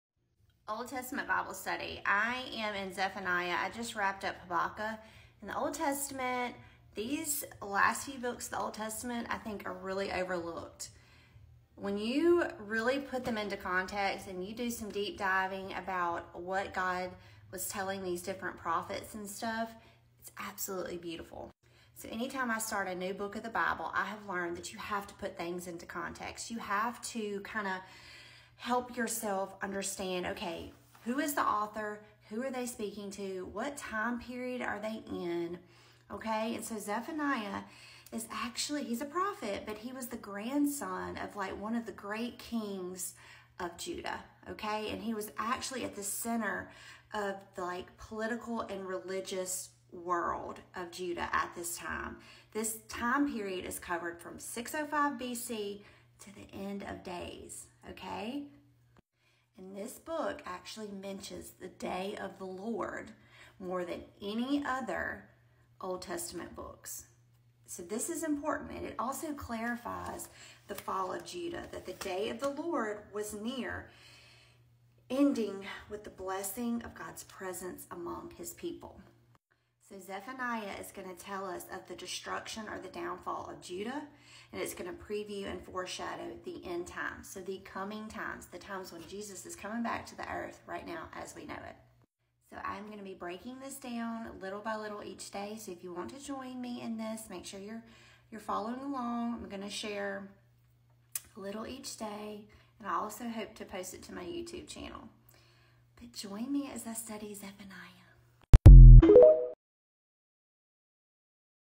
Old Testament Bible Study: Zephaniah
Let’s study the Old Testament book of Zephaniah. A book that tells us of the coming of the Lord. There’s many similarities in the sins of this time period in Judah to today’s world.

Remember to research context and apply it to this book.